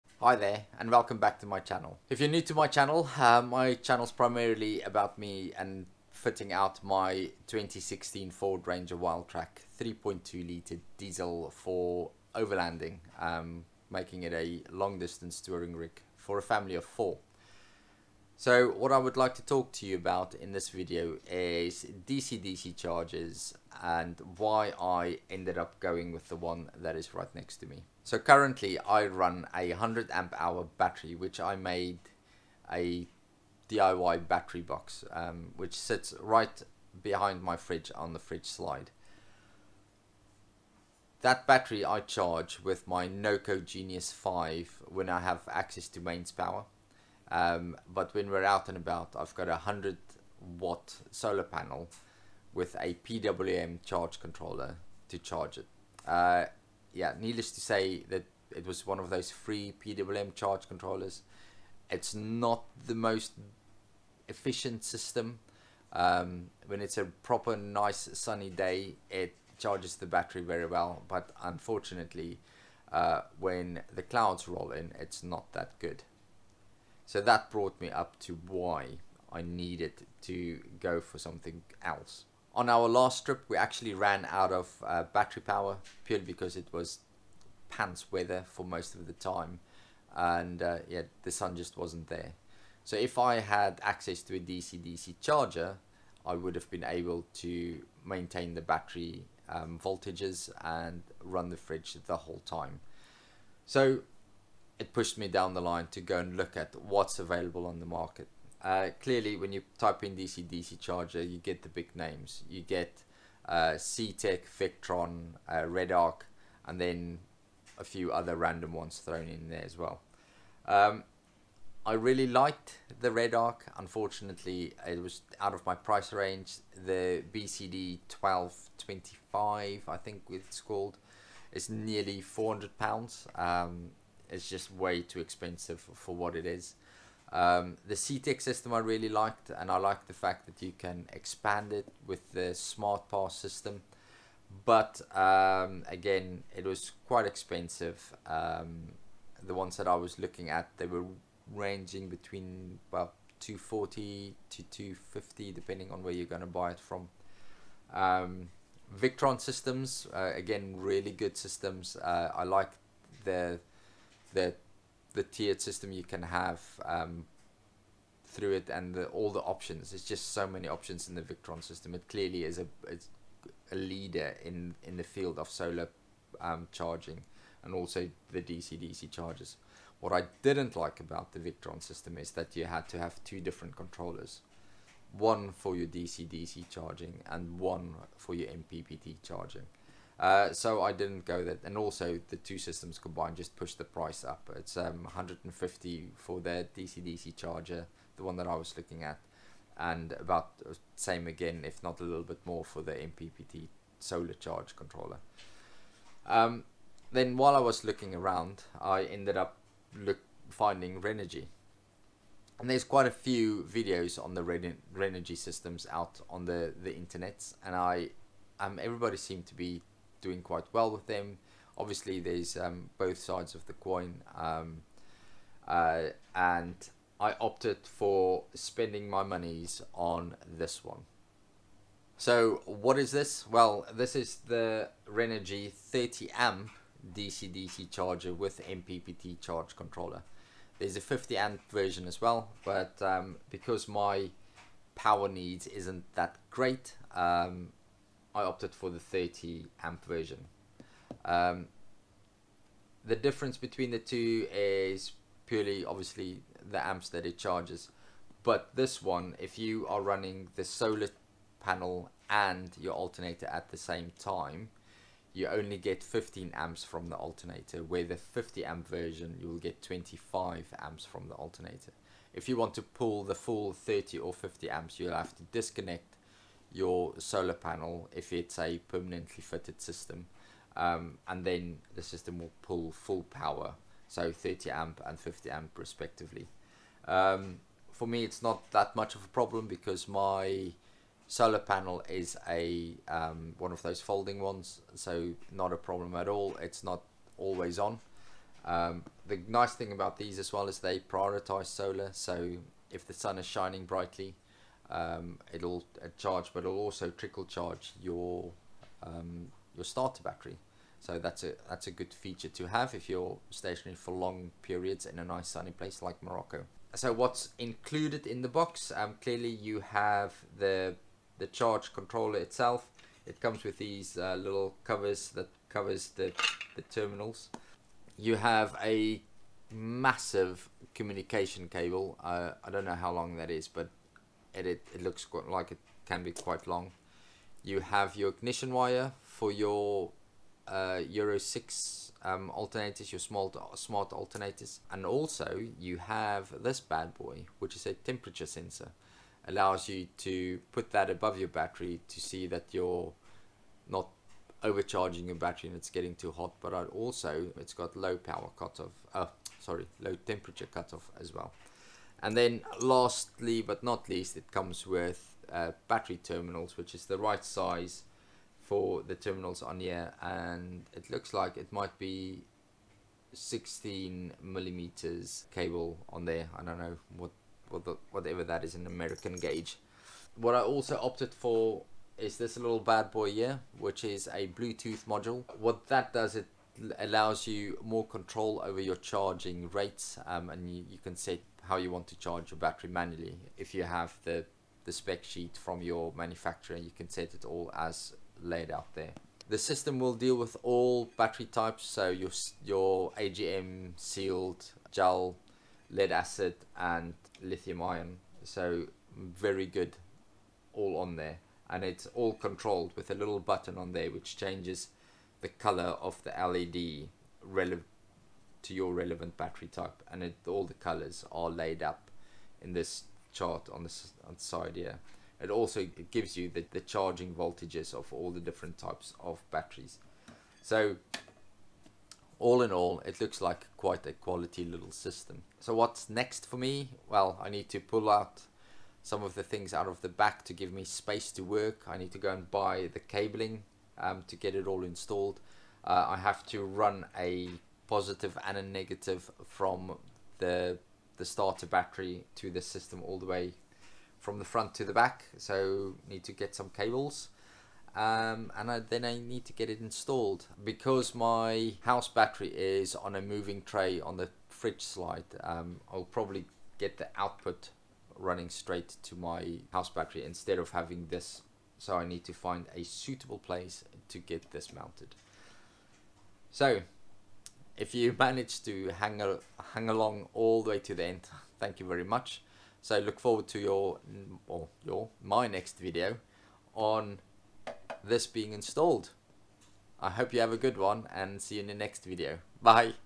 Reasons Behind My Selection of the Renogy DCC30S 12V 30A DC-DC Battery Charger With MPPT Solar Input
Talking head video about why I chose to go with the Renogy 30A DC-DC charger with MPPT Solar charge controller.

RENOGY
30A DC-DC w/MPPT Controller
50A DC-DC w/MPPT Controller

00:00 Intro
01:20 Why I need a DC-DC Charger
01:45 Alternatives on the Market
03:30 Why I chase RENOGY
03:50 Features of the System
05:25 What do you get in the box?
06:20 BT-2 overview
07:15 What Next?
08:00 Closing